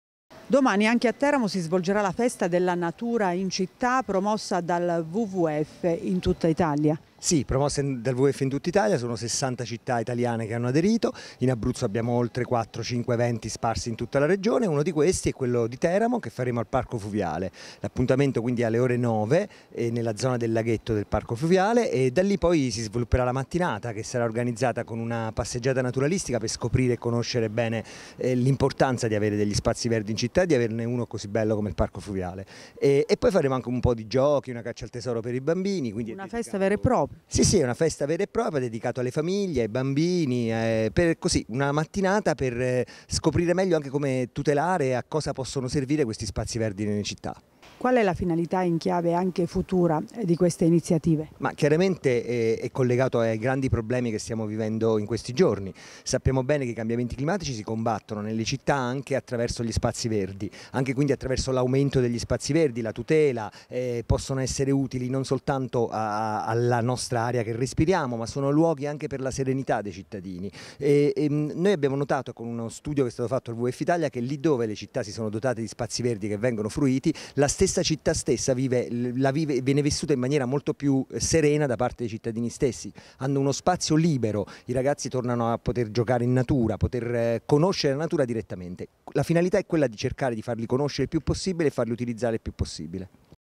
Teramo, per "Urban Nature" domani il Wwf porta alla scoperta del parco fluviale del Vezzola
| SUPER J |

Contatti Redazione:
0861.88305